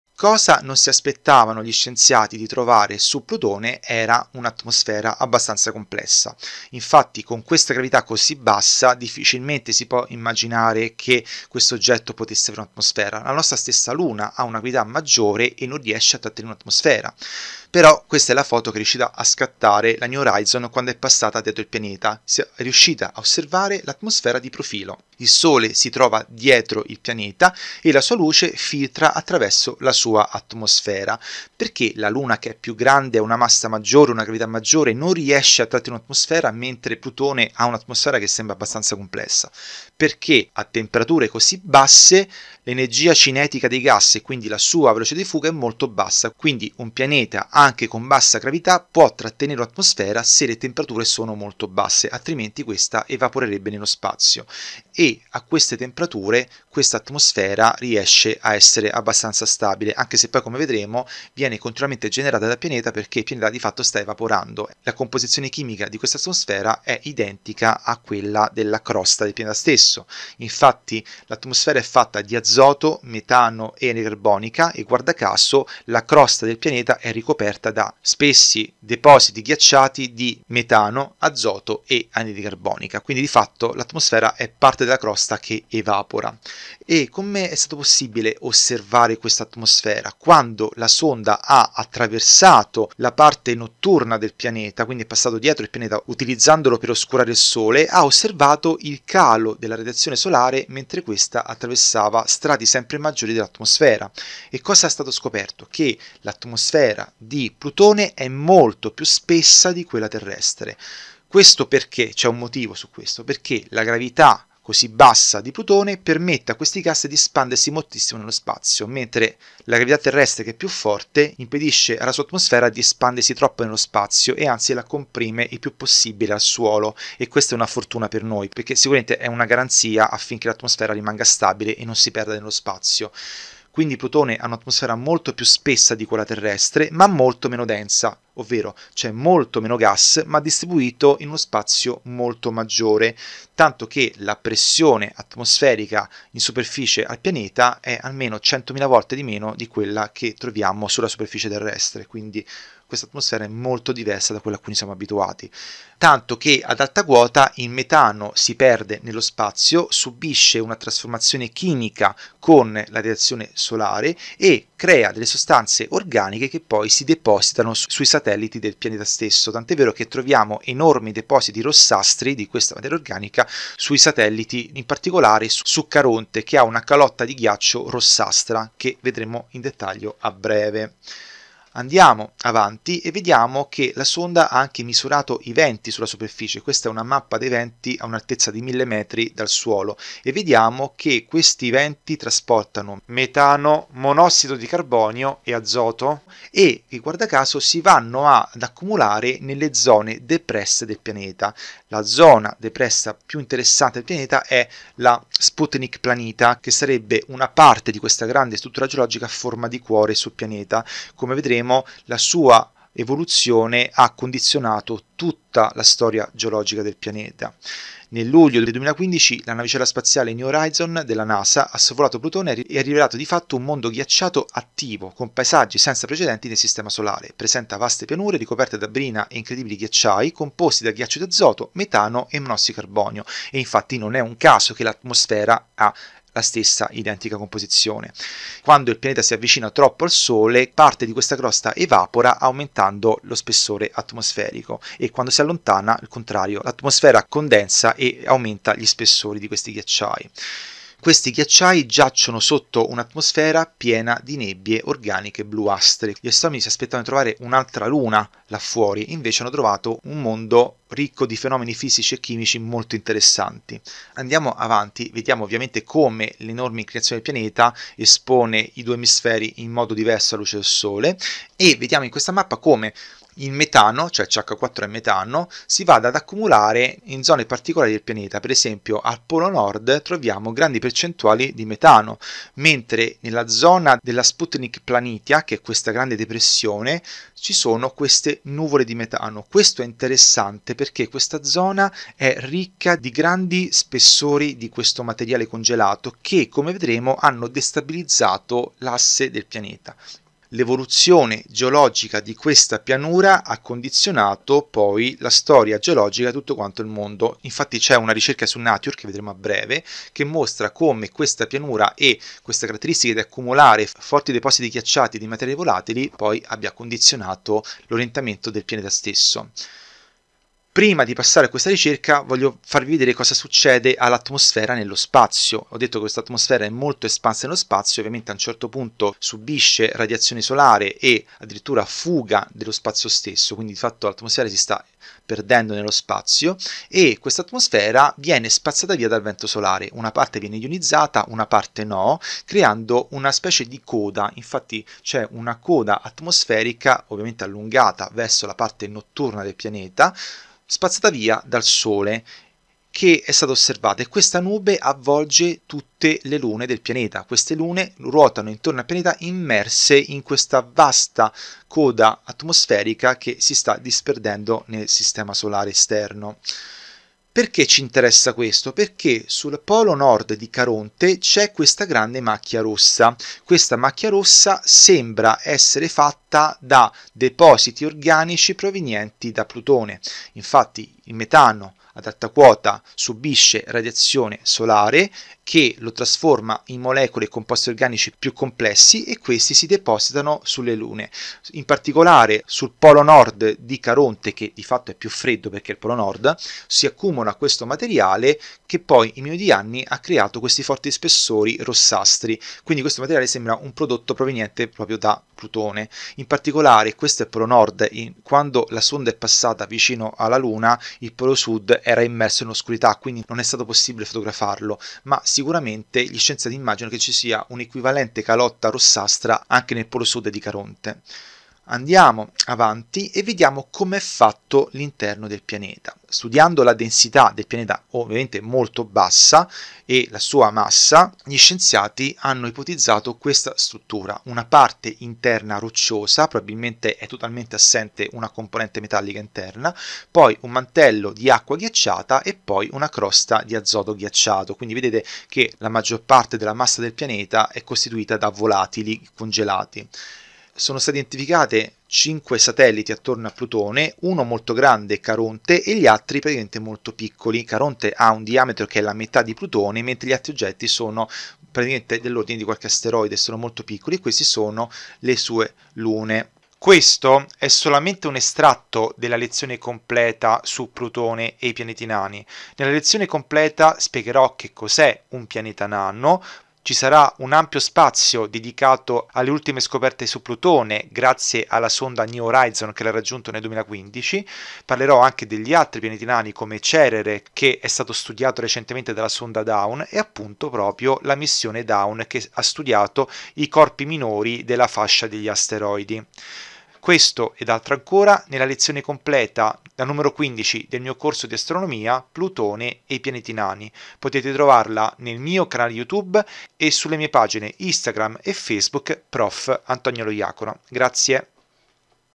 L'ATMOSFERA DI PLUTONE
In questo estratto spiego cosa la New Horizon ha scoperto dell'inaspettata atmosfera di Plutone.

VISITA IL MIO CANALE YOUTUBE

Prof. Antonio Loiacono

Prof. Antonio Loiacono